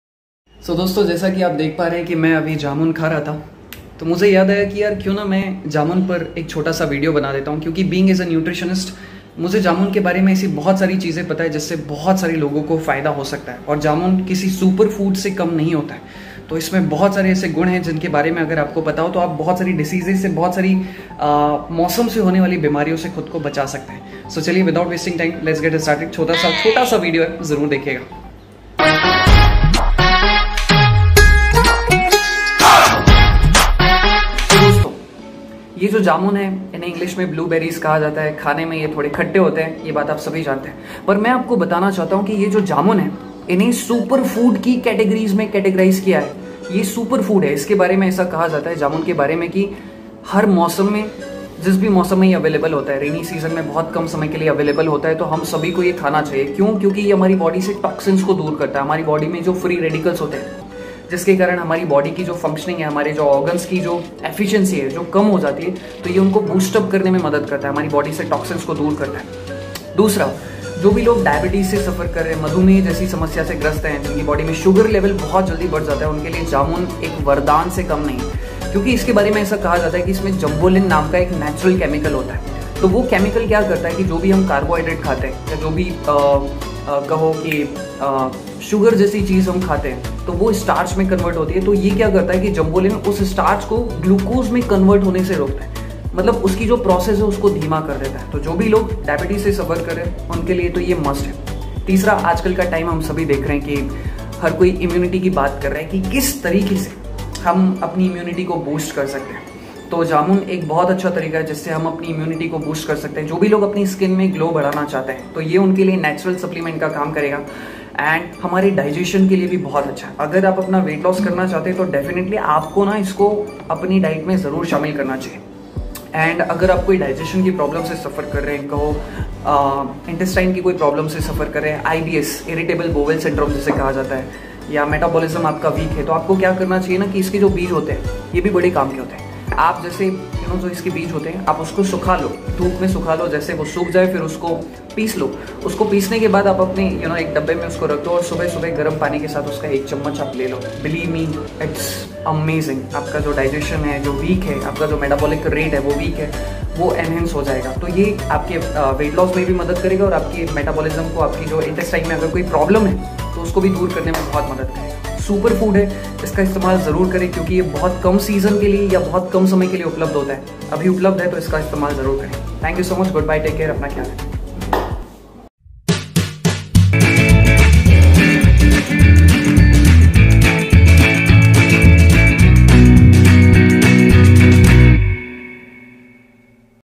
Benefits Of Eating Blue-Berries/(जामुन)🔥(Metabolism,Weight loss,detox,Immunity) Superfood👍
Jaamun/Blueberries are best source
of Vitamins and minerals..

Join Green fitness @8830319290